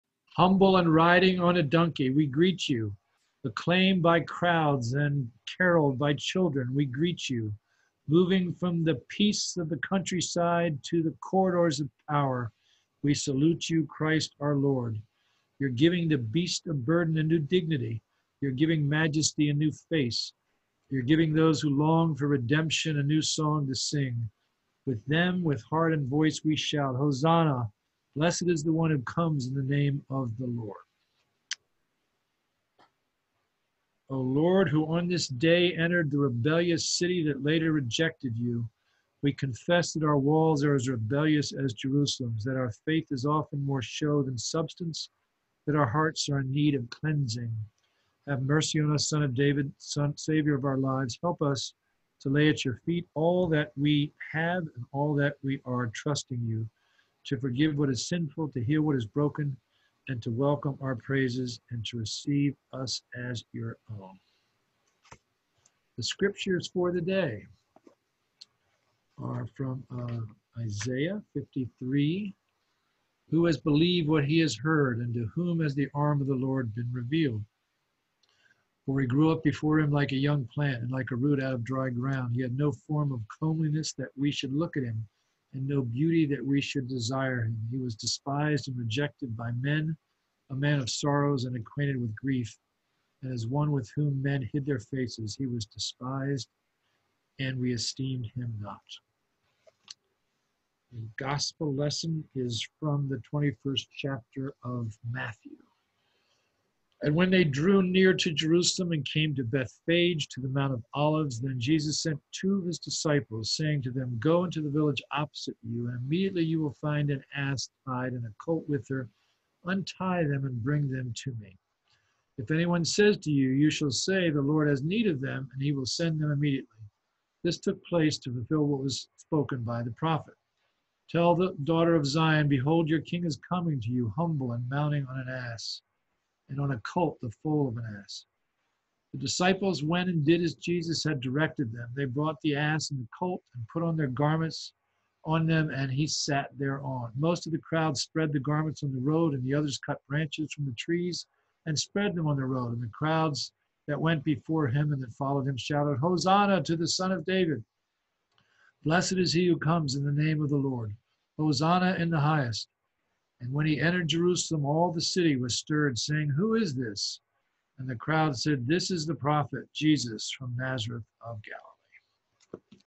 Rev. Taylor Readings for Palm Sunday 2020
Our first CCB Covid-19 recording with Rev. Taylor.  Thank you to Jackie Harris-Stone, Music Director, for guiding Rev. Taylor through the technology! This video contains the following: 1) Opening Prayer for Palm Sunday; 2) Prayer of Confession; 3) First Reading from Isaiah 53 1:3; and 4) Gospel Reading Matthew 21 1:11